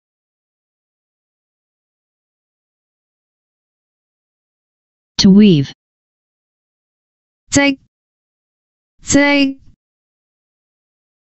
"TO WEAVE" in Cantonese (織) - Flashcard
[jik1] "TO WEAVE" in Cantonese (織) - Flashcard. Additional information about the word: to weave,to knit.